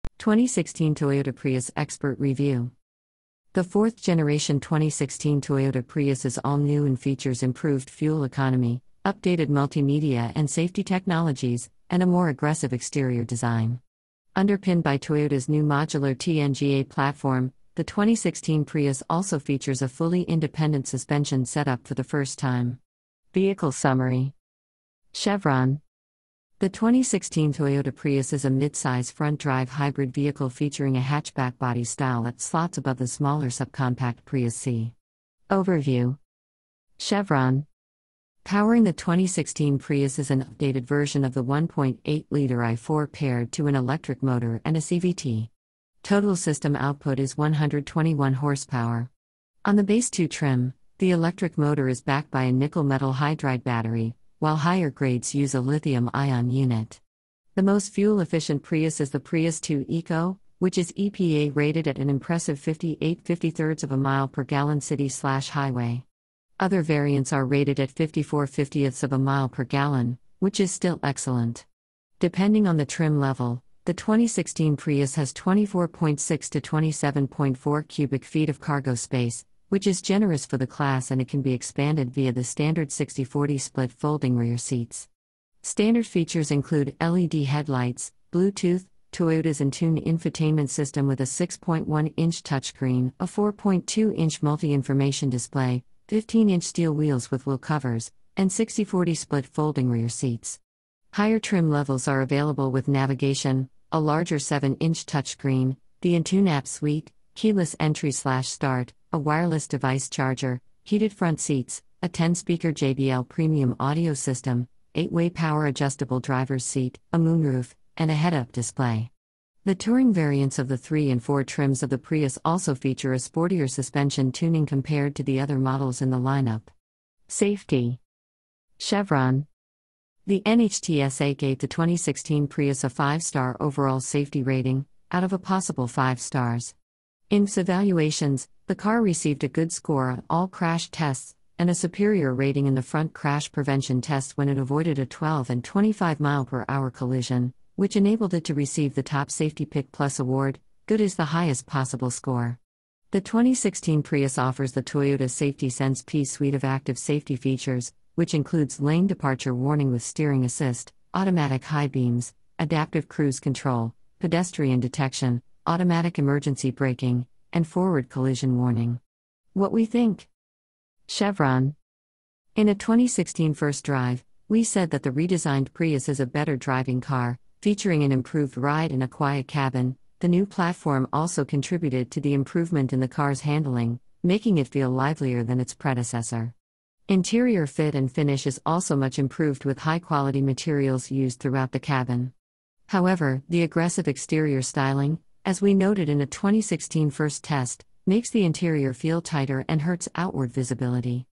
2016 Prius English Us
2016 Prius English Us
2016 Toyota Prius Expert Review
The fourth-generation 2016 Toyota Prius is all-new and features improved fuel economy, updated multimedia and safety technologies, and a more aggressive exterior design. Underpinned by Toyota's new modular TNGA platform, the 2016 Prius also features a fully independent suspension setup for the first time.
Vehicle Summary
chevron
The 2016 Toyota Prius is a midsize front-drive hybrid vehicle featuring a hatchback body style that slots above the smaller subcompact Prius C.
Overview
chevron
Powering the 2016 Prius is an updated version of the 1.8-liter I-4 paired to an electric motor and a CVT. Total system output is 121 hp. On the base Two trim, the electric motor is backed by a nickel-metal hydride battery, while higher grades use a lithium-ion unit. The most fuel efficient Prius is the Prius Two Eco, which is EPA-rated at an impressive 58/53 mpg city/highway. Other variants are rated at 54/50 mpg, which is still excellent. Depending on the trim level, the 2016 Prius has 24.6 to 27.4 cubic feet of cargo space, which is generous for the class and it can be expanded via the standard 60/40 split-folding rear seats.
Standard features include LED headlights, Bluetooth, Toyota's Entune infotainment system with a 6.1-inch touchscreen, a 4.2-inch multi-information display, 15-inch steel wheels with wheel covers, and 60/40 split-folding rear seats. Higher trim levels are available with navigation, a larger seven-inch touchscreen, the Entune app suite, keyless entry/start, a wireless device charger, heated front seats, a 10-speaker JBL premium audio system, eight-way power adjustable driver's seat, a moonroof, and a head-up display. The Touring variants of the Three and Four trims of the Prius also feature a sportier suspension tuning compared to the other models in the lineup.
Safety
chevron
The NHTSA gave the 2016 Prius a five-star overall safety rating (out of a possible five stars). In IIHS evaluations, the car received a Good score on all crash tests and a Superior rating in the front crash prevention test when it avoided a 12- and 25-mph collision, which enabled it to receive the Top Safety Pick+ award (Good is the highest possible score). The 2016 Prius offers the Toyota Safety Sense-P suite of active safety features, which includes lane departure warning with steering assist, automatic high beams, adaptive cruise control, pedestrian detection, automatic emergency braking, and forward collision warning.
What We Think
chevron
In a 2016 First Drive, we said that the redesigned Prius is a better driving car, featuring an improved ride and a quiet cabin, The new platform also contributed to the improvement in the car's handling, making it feel livelier than its predecessor. Interior fit and finish is also much improved with high-quality materials used throughout the cabin. However, the aggressive exterior styling, as we noted in a 2016 First Test, makes the interior feel tighter and hurts outward visibility.